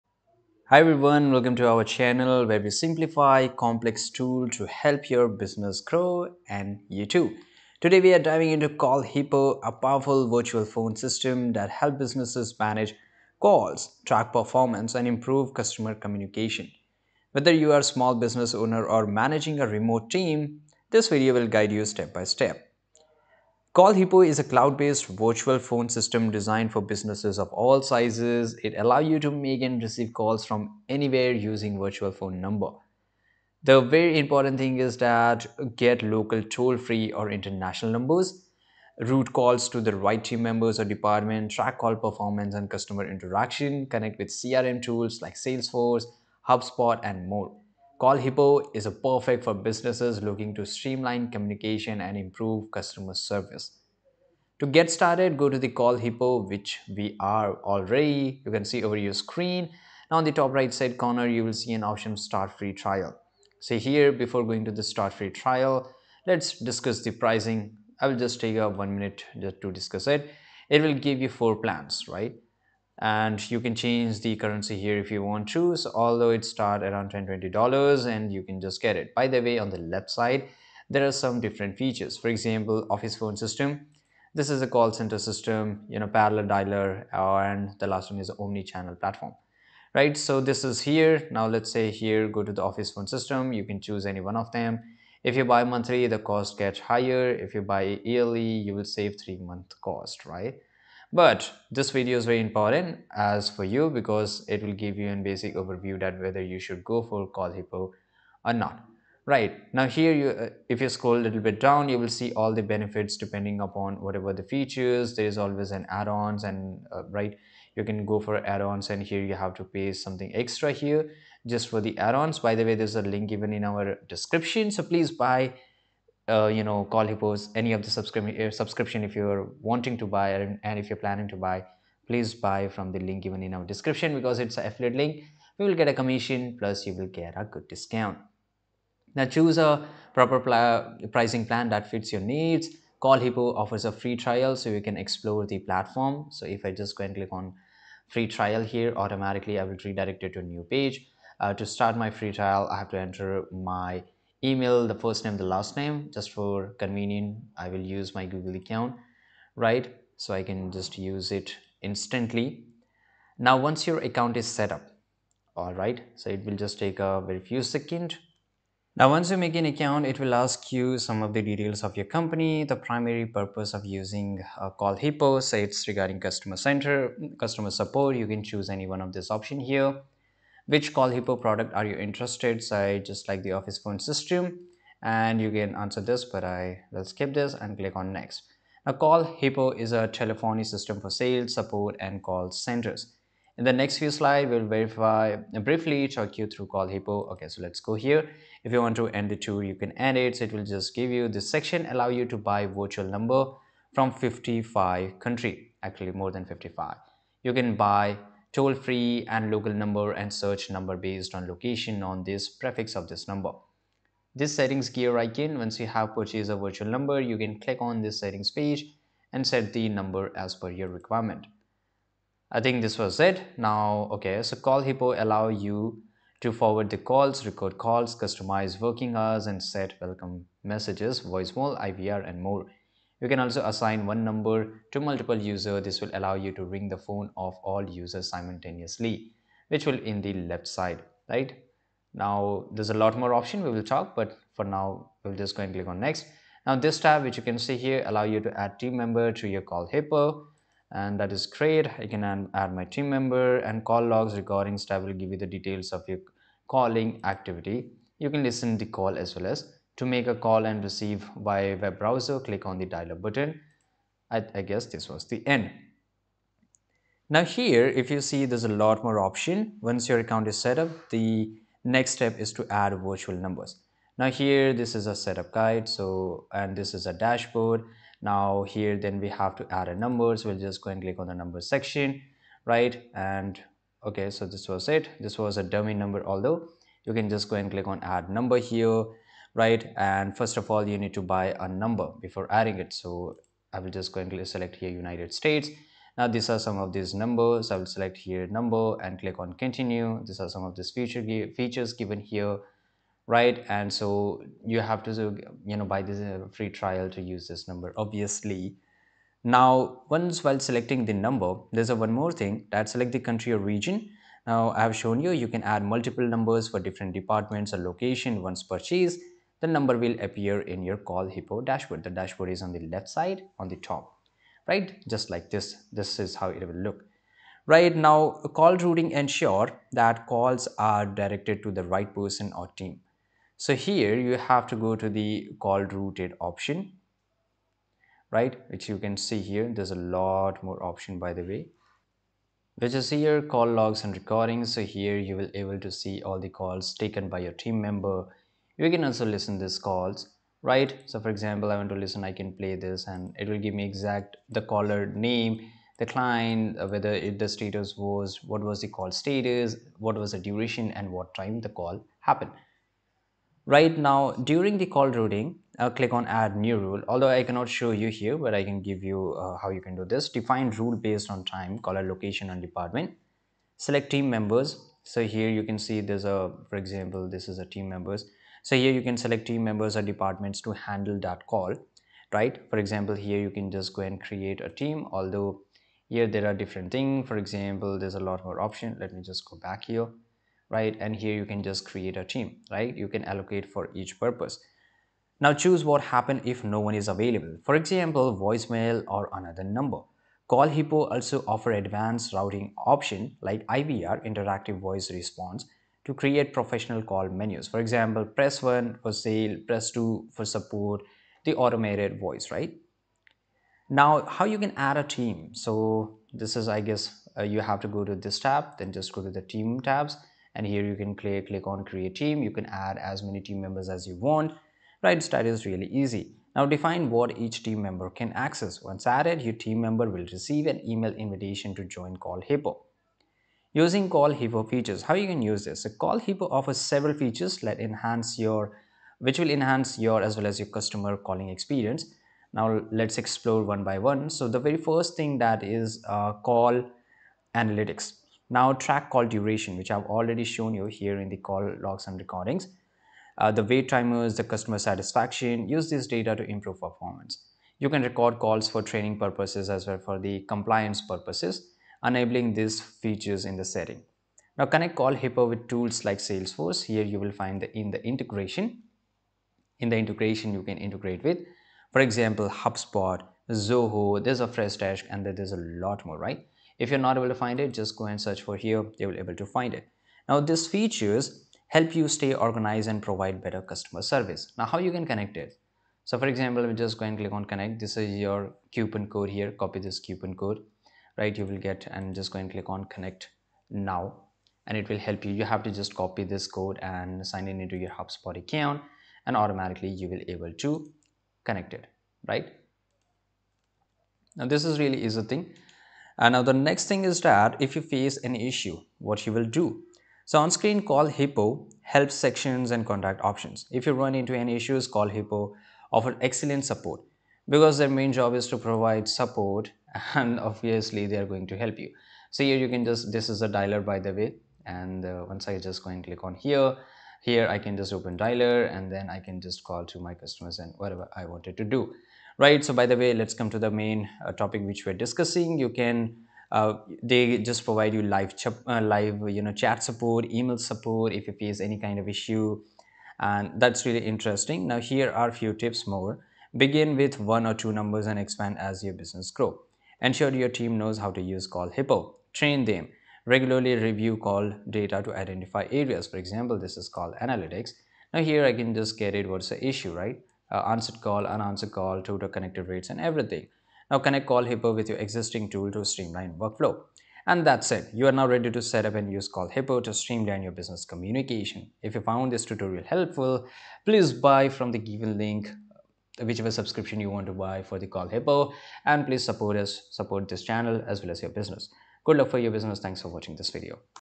CallHippo Tutorial for Beginners | Step-by-Step Guide to Setting Up CallHippo (New)
Looking to streamline your business communication in ? This CallHippo Tutorial for Beginners provides a comprehensive, step-by-step guide to setting up CallHippo , the leading cloud telephony platform . Whether you're managing a startup or scaling a large team, learn how to leverage VoIP systems and virtual phone numbers to enhance your call management and customer support workflows. From IVR setup to seamless CRM integration , this guide ensures you maximize the potential of CallHippo's call center software . Discover the tools for efficient call routing , real-time call analytics , and scalable solutions designed for remote work environments. Perfect for beginners, this tutorial will help you master the user-friendly interface and unlock the full power of business phone systems tailored for modern enterprises.